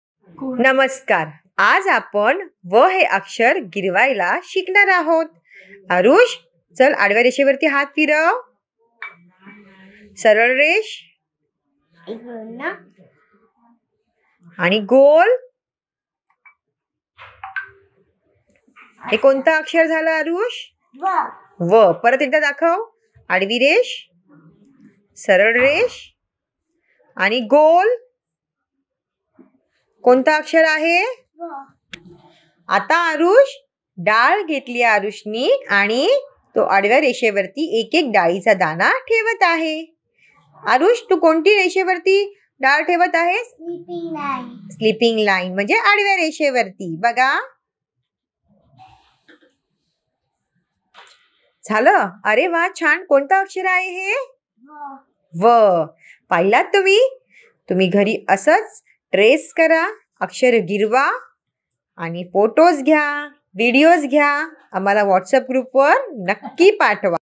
धान्याने आणि बोटाने "व" रेखाटने. | Tracing letters व with finger and grains | मराठी | (3-6 years)
Both(Pre-Primary I (3-4 years) and Pre-Primary II (5-6 years)) वयातील मुलांचे भाषा विकास सुधारण्यासाठी त्यांच्याकडून Activity करवून घ्या।